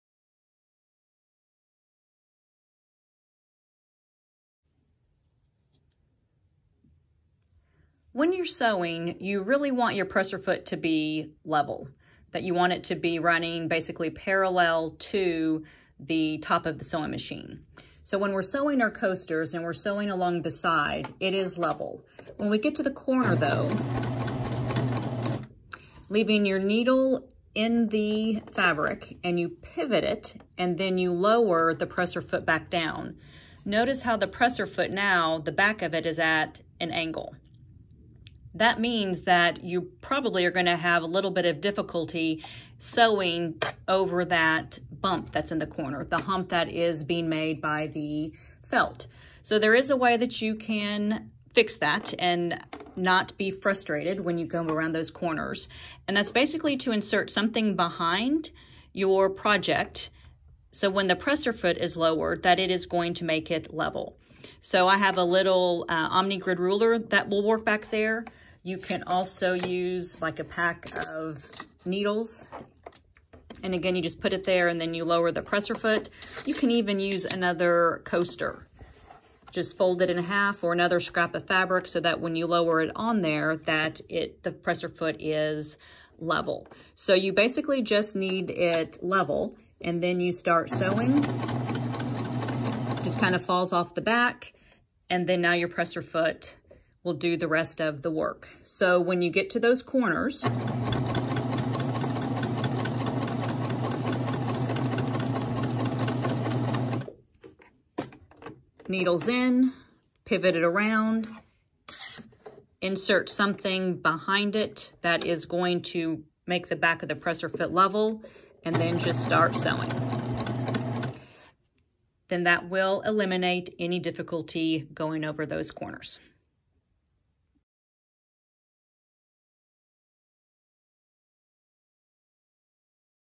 Hack - How to Sew Over Corner Humps
Have you ever pivoted around a corner only to find yourself frustrated because the feed dogs struggle to move the fabric forward? Never fear, there is a solution. See how easy it easy to solve this problem.

This hack is part of the blog post on how to sew fabric drink coasters. See the full article